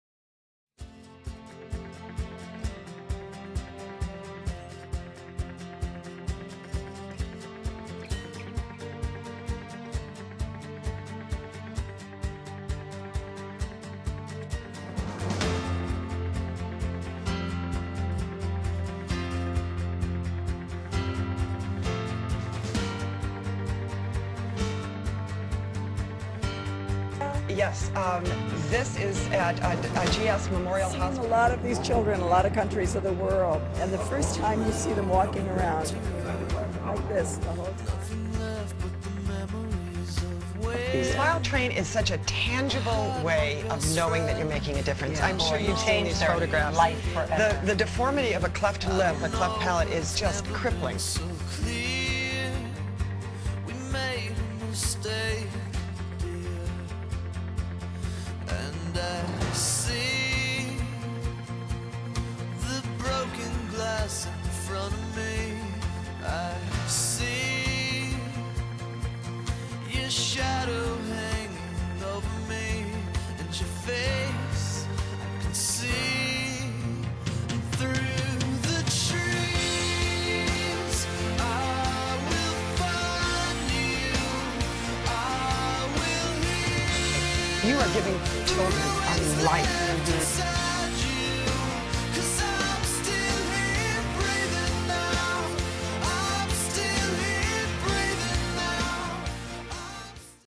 SmileTrain. Children with cleft lip
Had to make a video for World History, this was the product. Enjoy : ) Sorry the sounds doesn't work :/